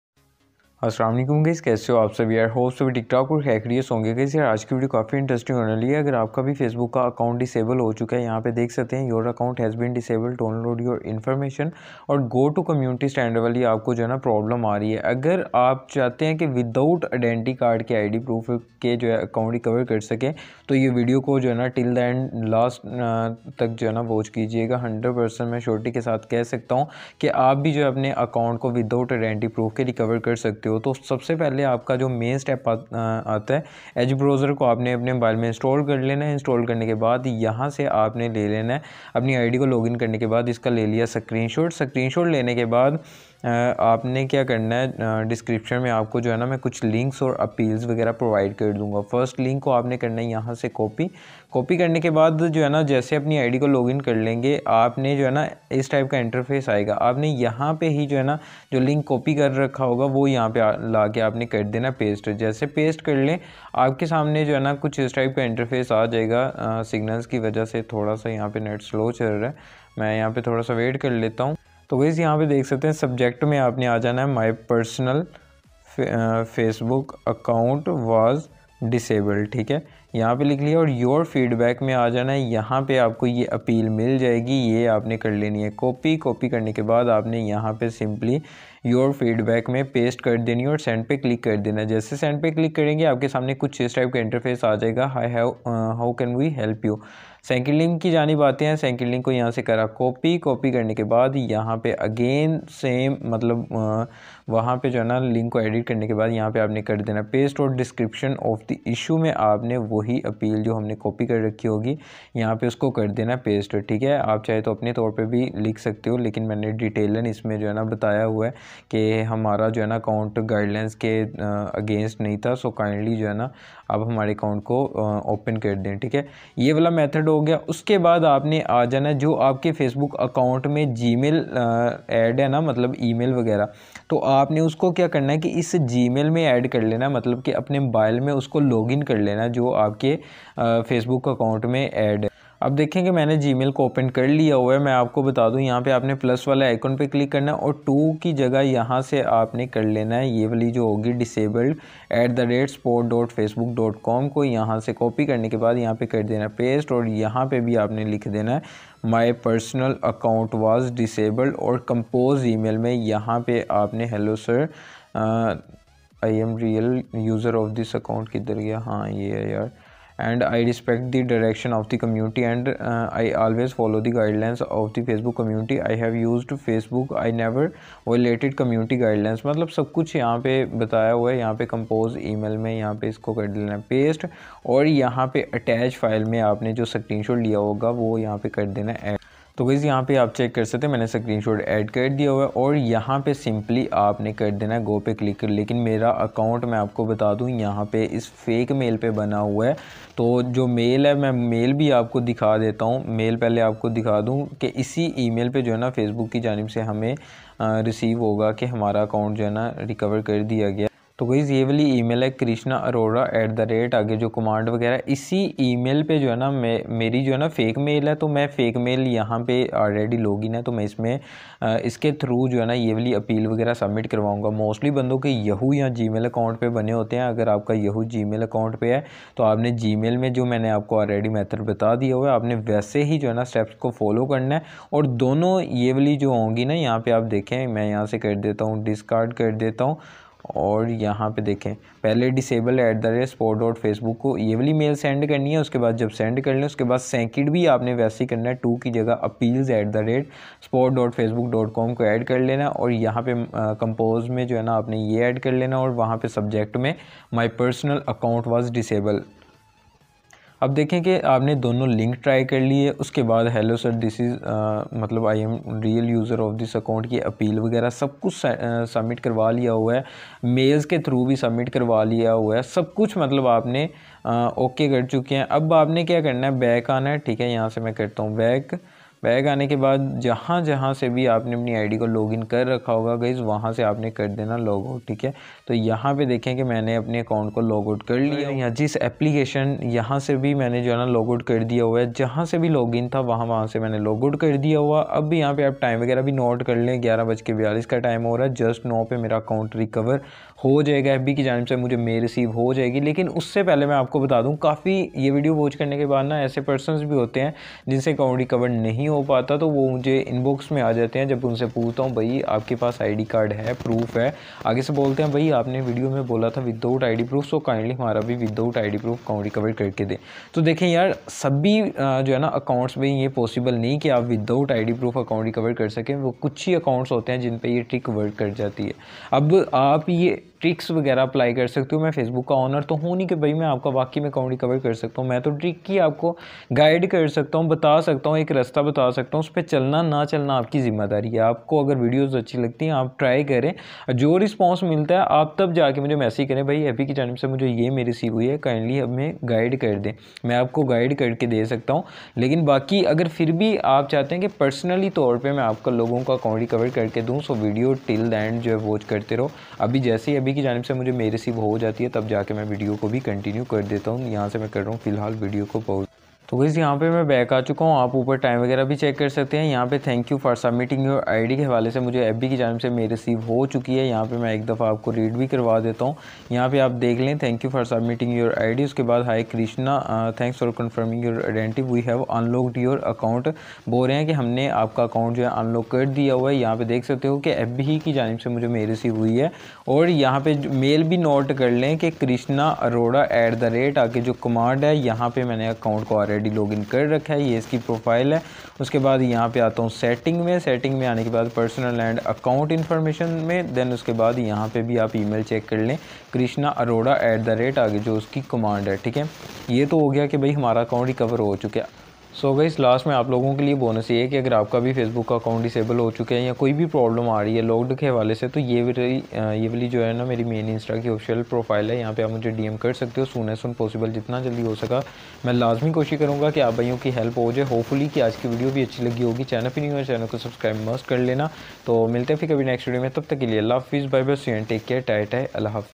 Without id proof Disabled account recover | how yo recover facebook disabled account 2022
Hello sir I’m real user of this account and I respect the direction of the community and I always follow the guidelines of the Facebook community. I’ve used Facebook I’ve never violated community guidelines, your Facebook protection has been accidentally Uplod your id my account. But I’m provide you all identity with is enough to reopen my Facebook account i hope you look my appeal and open my Facebook account as soon as possible, Thanks.

🎁Thanks for your love and Support.

🎋Keep Supporting 💪🏻
💡Keep Loving ❤
✨Keep Sharing ⤴